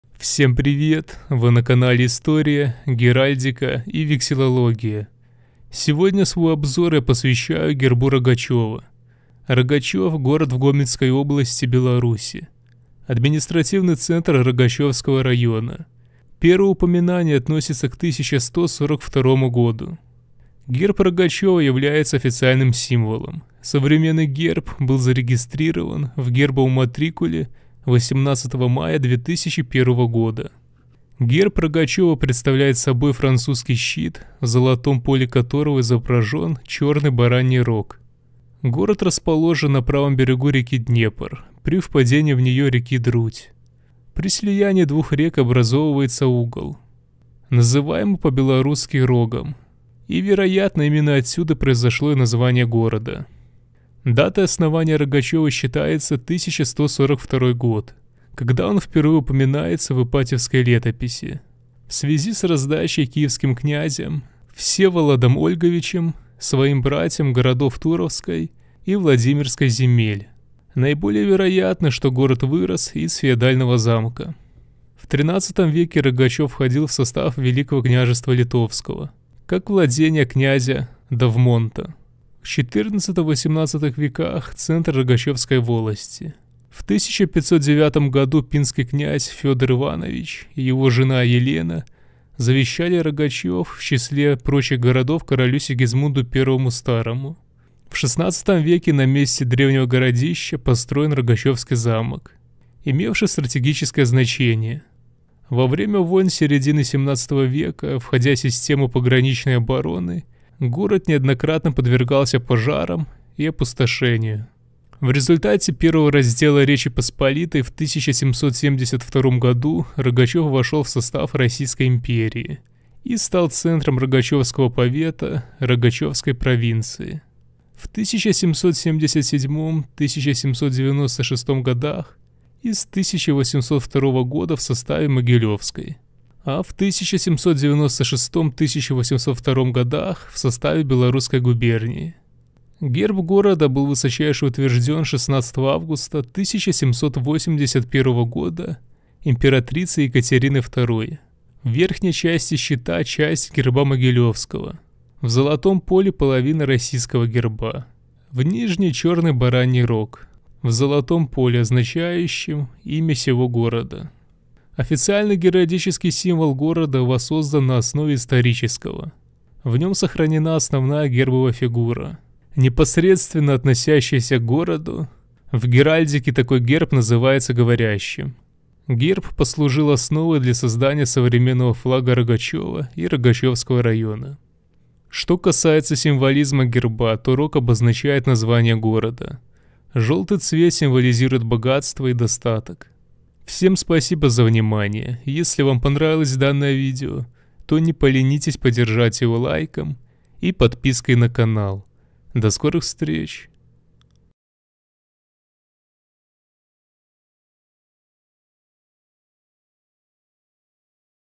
Герб Рогачёва. Беларусь.
Coat of arms of Rogachev. Belarus. В этом видео Вы узнаете о гербе Рогачёва, его описание, историю и значение. И конечно же символизм элементов изображённых на нём. Республика Беларусь.